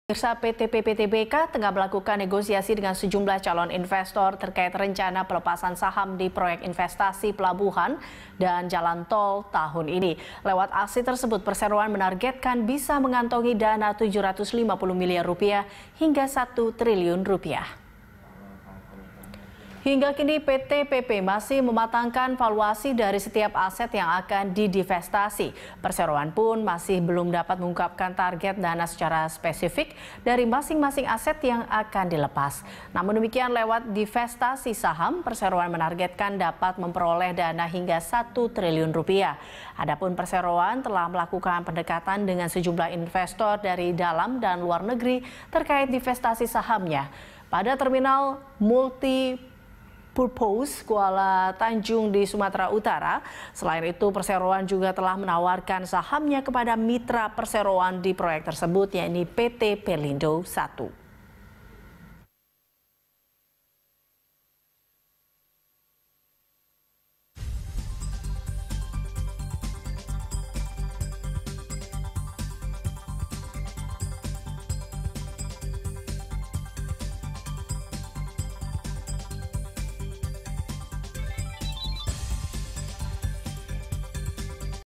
Divestasi Saham, PP Targetkan Kantongi Dana Hingga Rp1 Triliun
PT PP Tbk tengah melakukan negosiasi dengan sejumlah calon investor terkait rencana pelepasan saham di proyek investasi pelabuhan dan jalan tol tahun ini. Lewat aksi tersebut perseroan menargetkan bisa mengantongi dana Rp 750 miliar hingga Rp 1 triliun.

Facebook.com/BeritaSatu
Youtube.com/BeritaSatu